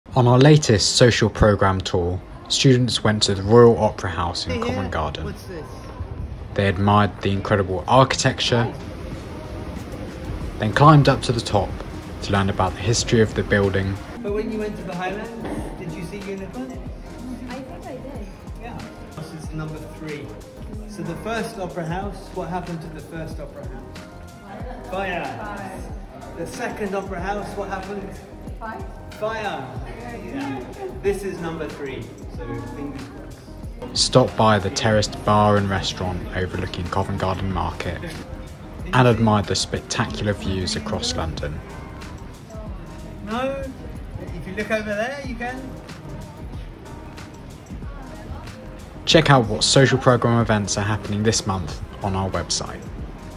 Visit to Royal Opera House 🎭⁠
On our latest Social Programme tour, James took students to the Royal Opera House. They looked at the incredible architecture, learned about the history of the building, stopped by the terraced bar and restaurant overlooking Covent Garden and admired the spectacular views across London. ⁠